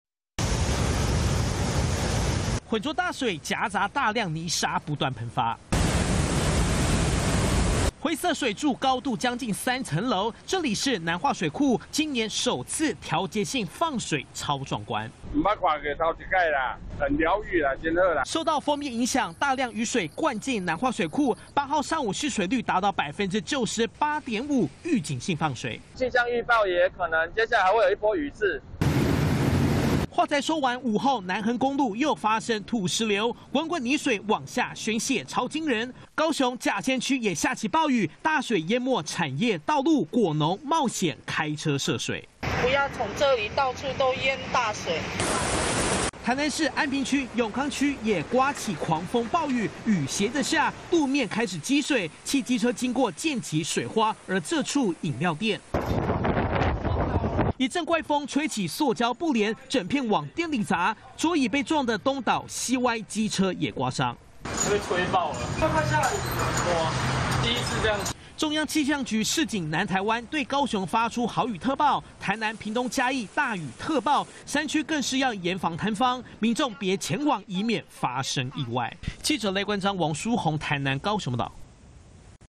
南台灣暴雨怪風! 南化水庫洩洪.南橫爆土石流│中視新聞 20220608
梅雨鋒面來襲，南台灣今天颳風又下大雨，南化水庫，預警性洩洪，灰色水柱高達3層樓高，高雄山區降下大雨，道路被大水淹沒，南橫公路也發生土石流，台南市區，不僅有雨還颳起怪風，飲料店塑膠帆布，砸倒騎樓下，一整排桌椅。